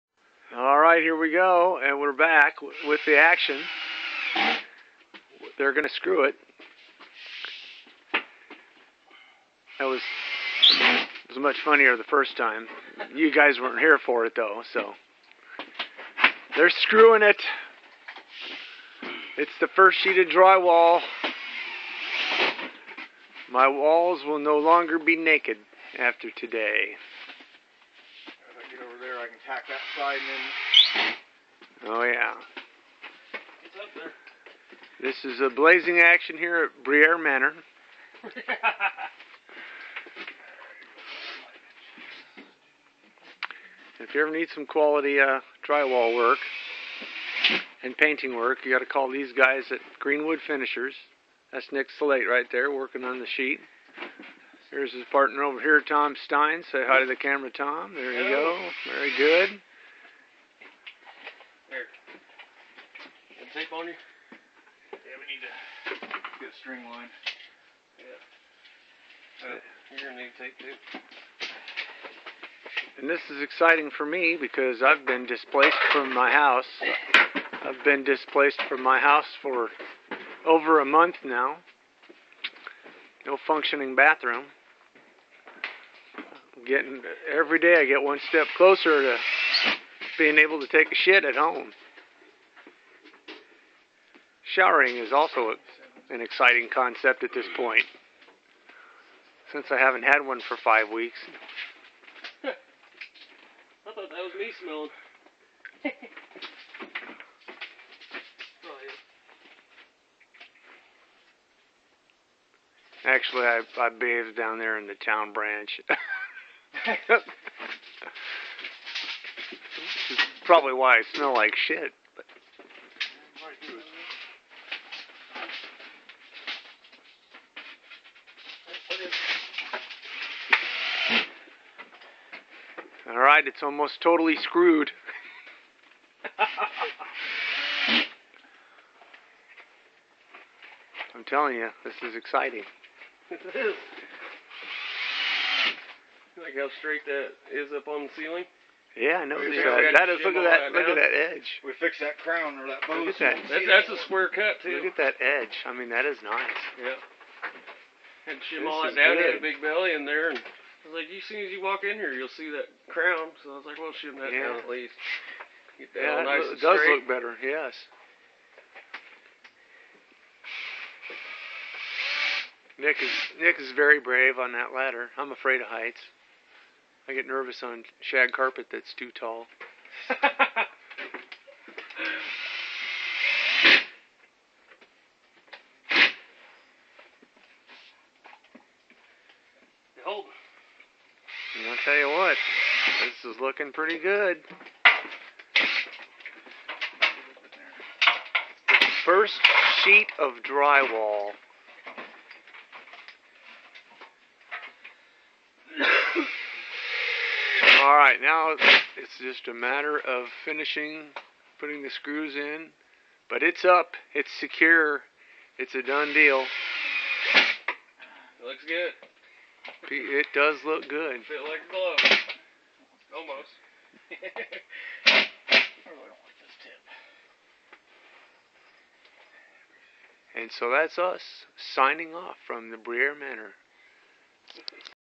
Bathroom Remodeling 2015-01-13 First Sheet of Drywall Part 2
This sheet of drywall gets totally screwed. I'm one step closer to being able to take a shit at home. Showering is an exciting concept, too, since I haven't had one for five weeks. Just kidding. I bathe down at the town branch.